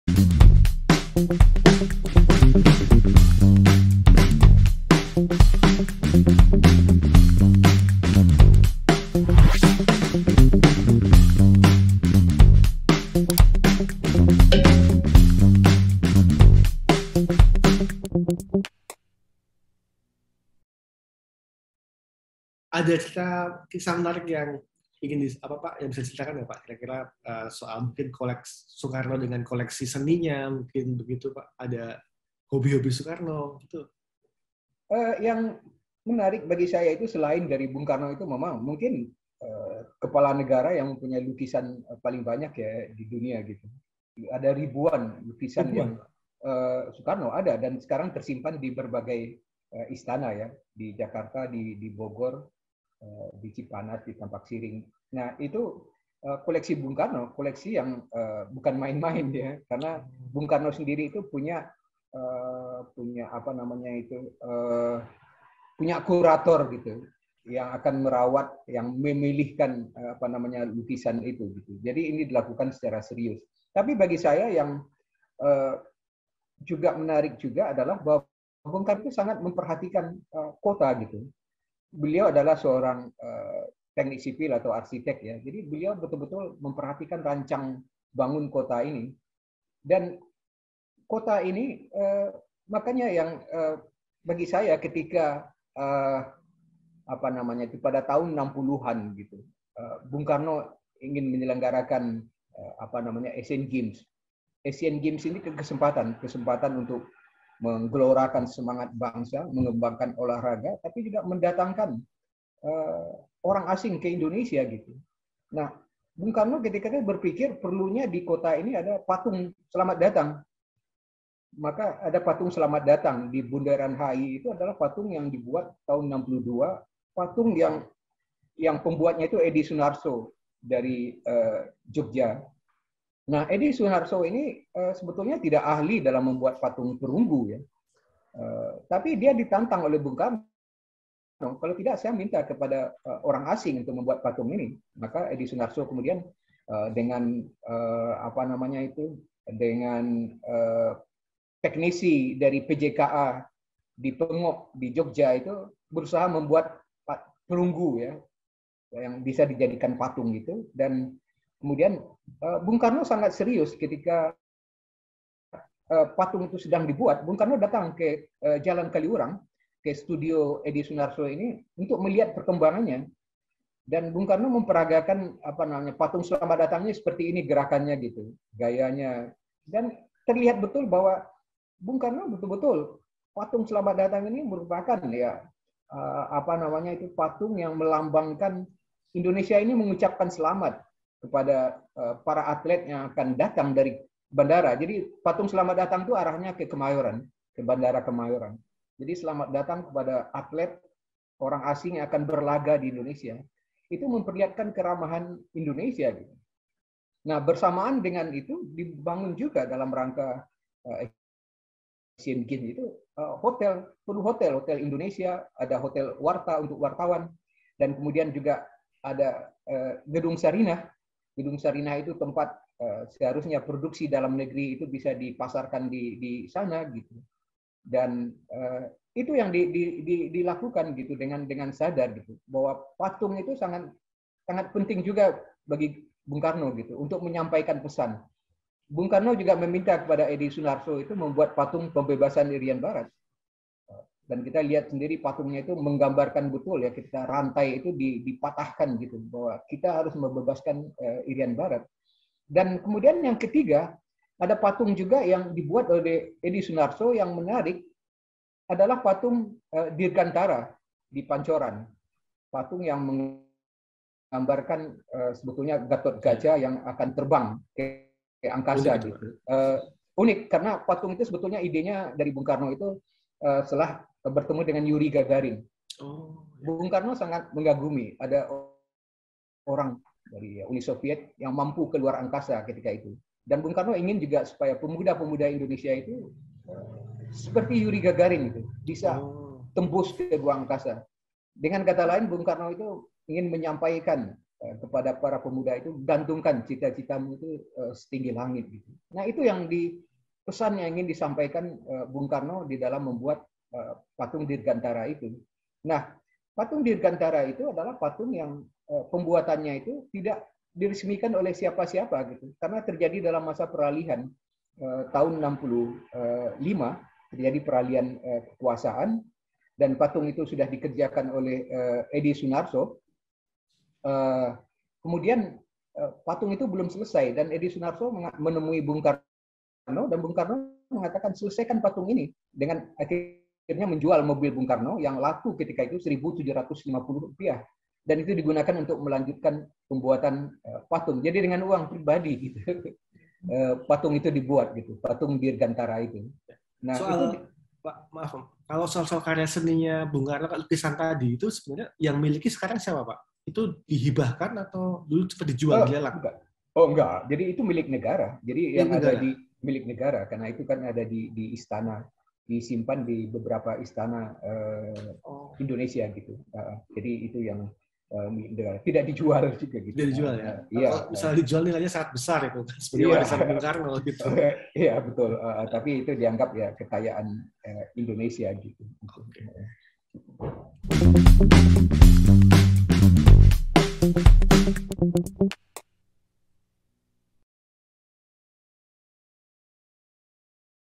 Koleksi Lukisan Soekarno-Asvi Warman Adam | Seri Sejarah
Berikut ini adalah Seri Sejarah Penerbit Buku Kompas yang berisi tentang ulasan fenomena sejarah yang terjadi. Dalam episode 4 ini, Prof. Asvi Warman Adam bercerita mengenai kisah-kisah seputar koleksi lukisan dari Soekarno.